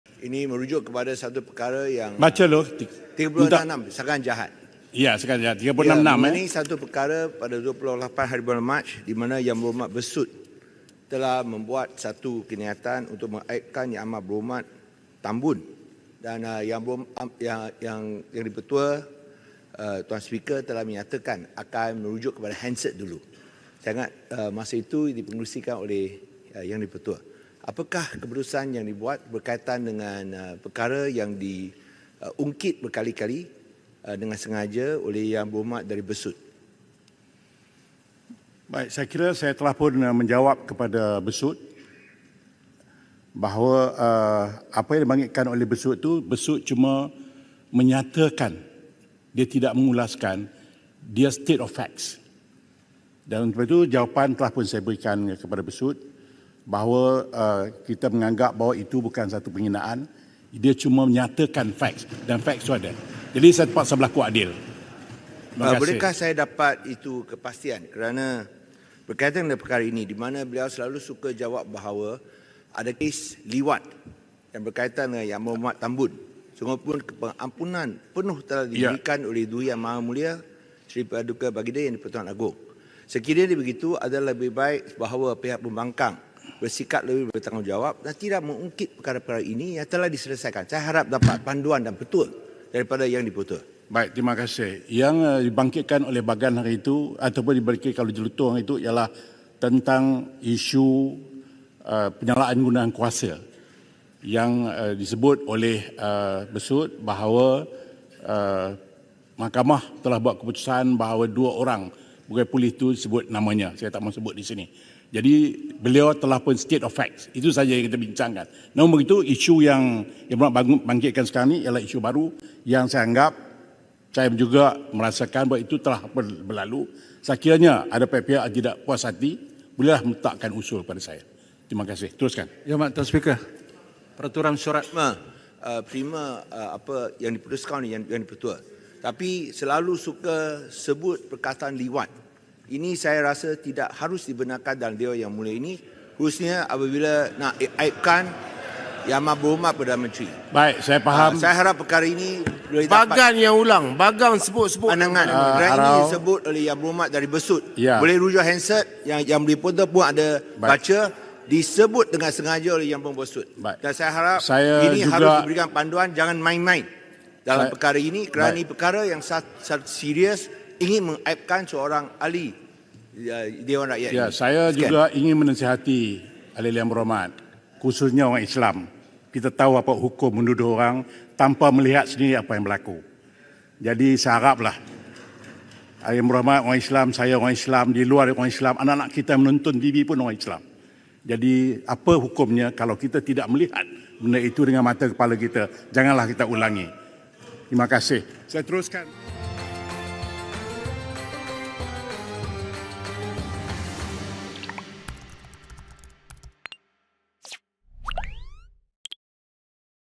Liwat: Jangan tuduh tanpa lihat sendiri, kata Johari
Speaker Dewan Rakyat Johari Abdul mengingatkan semua Ahli Parlimen beragama Islam tidak melemparkan tuduhan tanpa melihat sendiri apa yang berlaku, susulan perkataan “liwat” berulang kali dibangkit seorang Ahli Parlimen pembangkang.